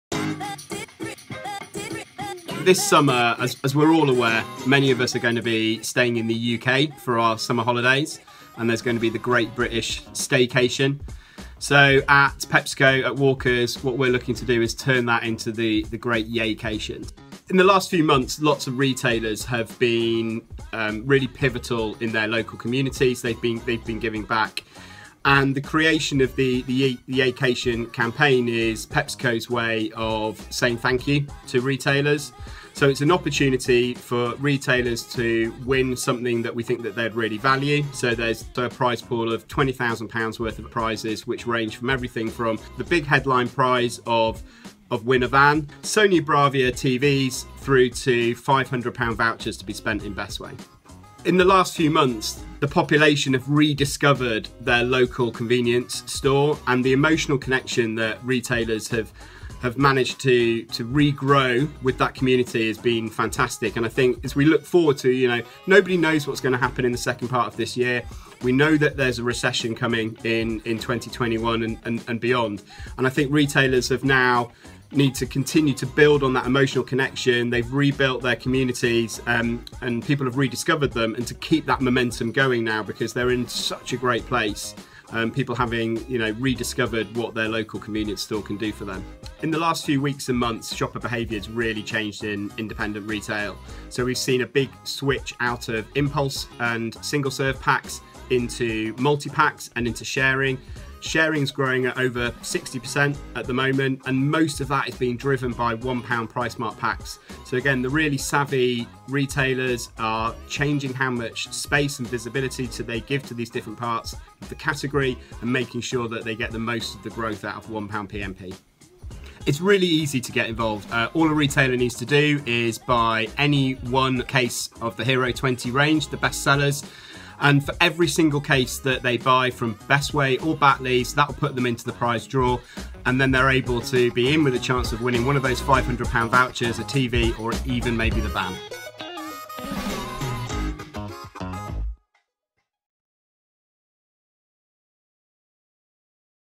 (Sponsored content) PepsiCo helping retailers put the ‘YAY’ in STAYCATION
(SPONSORED CONTENT) PepsiCo is helping to upgrade the nation’s staycations to YAY-cations with its biggest ever summer campaign, including a national retailer campaign with Bestway. The summer campaign will give retailers the chance to win a Ford Transit Courier Leader 1.5L, plus some great runner-up items, all part of a prize pool worth £20,000.

To enter, retailers simply need to purchase just one case of the Walkers Hero20 line-up to be automatically entered into the prize draw.